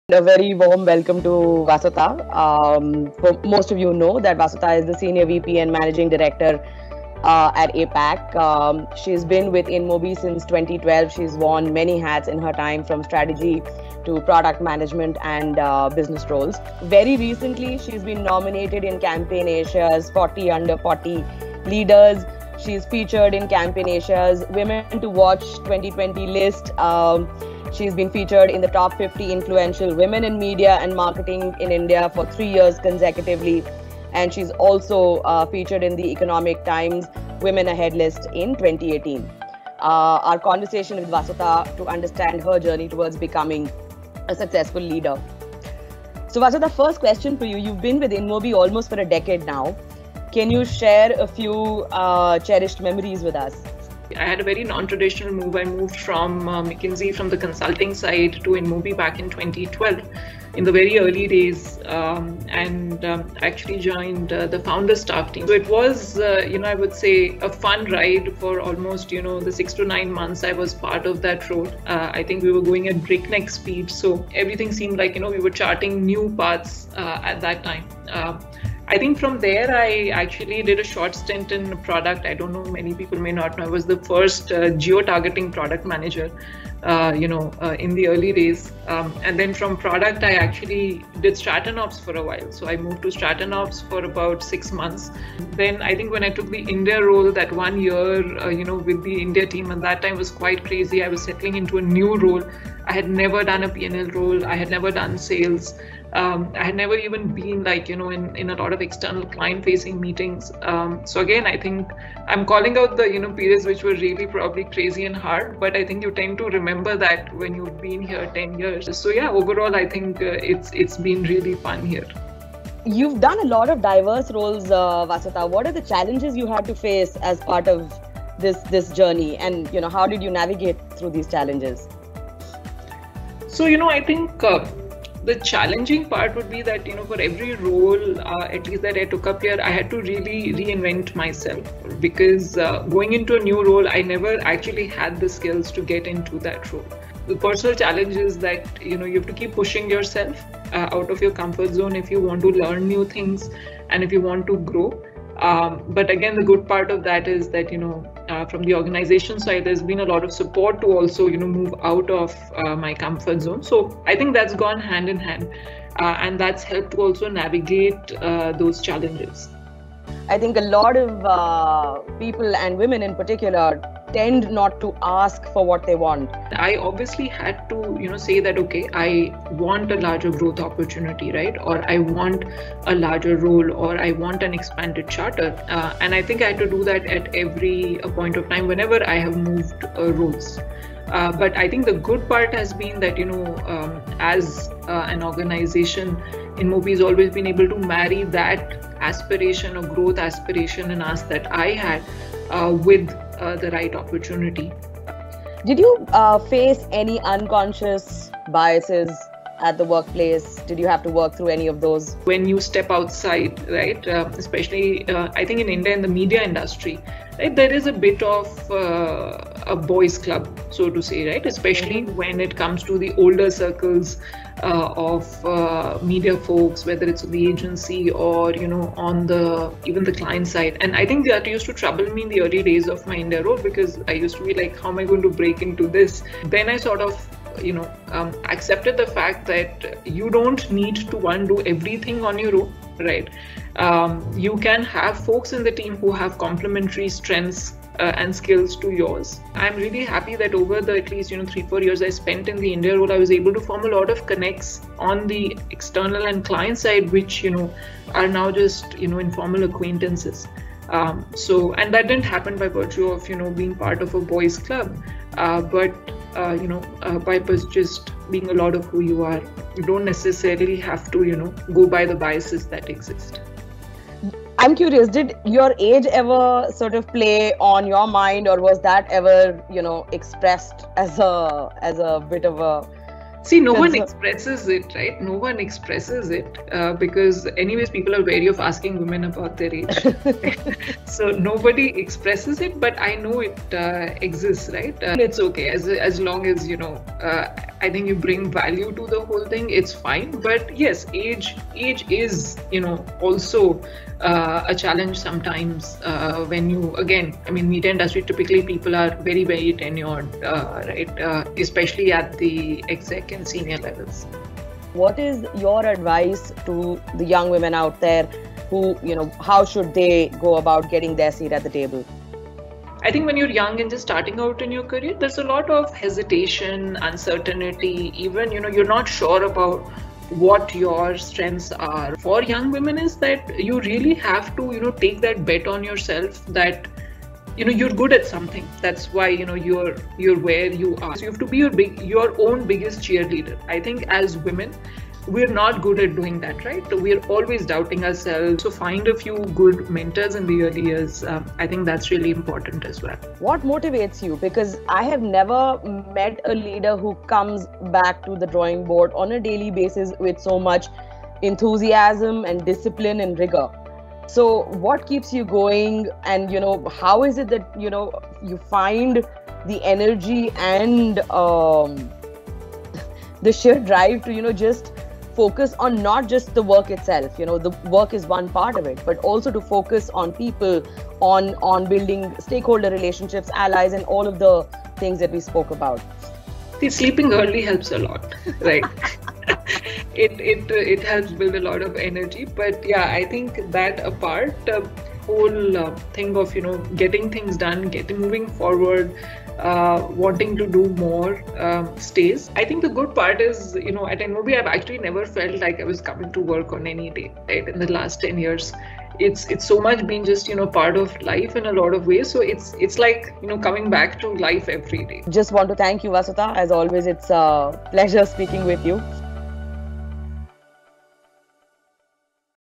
Winspire Spotlight Series: Vasuta Agarwal
Vasuta Agarwal, SVP and MD, Asia Pacific, at InMobi, is the Economic Times 40 under Forty 2021 leader known for being a champion of women’s leadership initiatives. She is also the executive sponsor in the APAC region for Winspire, InMobi’s employee resource group for women. In this conversation at Winspire’s Spotlight event, she opened up about her career journey and shared advice for women.